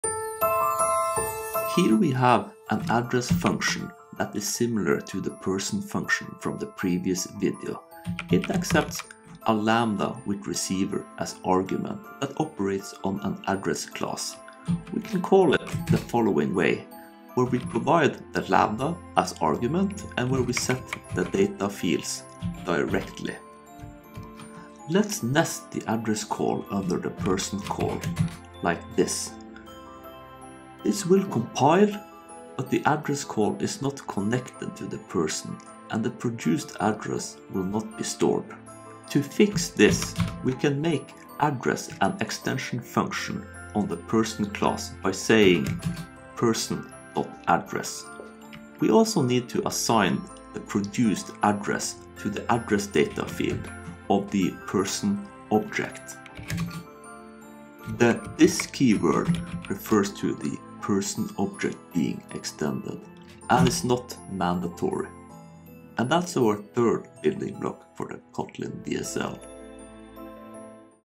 Luke 11 Kotlin DSL del 4
Luke 11 i JPro sin julekalender.

Kotlin DSL part 4 - Extension functions I “Nesting the Kotlin DSL by using extension functions”